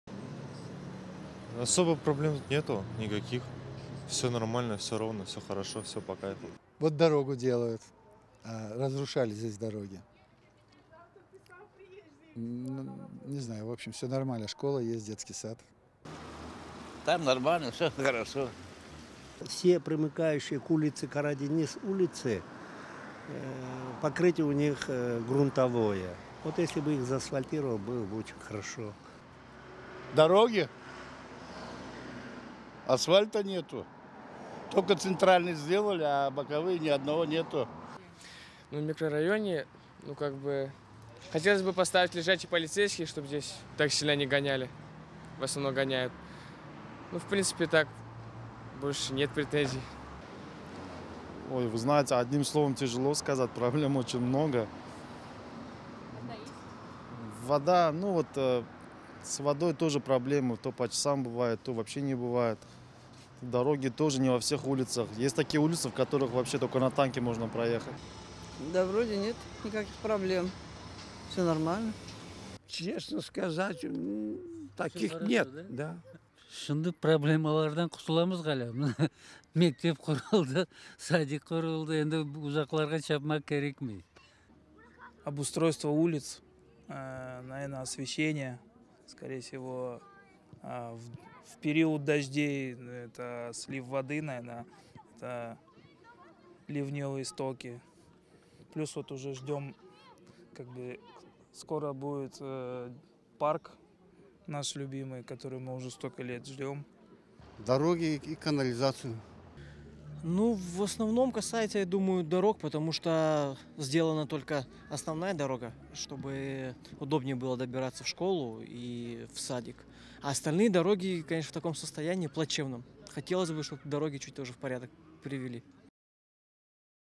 Какие проблемы в вашем микрорайоне вы считаете самыми острыми?
Мы поинтересовались у жителей одного из крымскотатарских микрорайонов, какие проблемы своего посёлка они считают самыми острыми. И вот что мы услышали.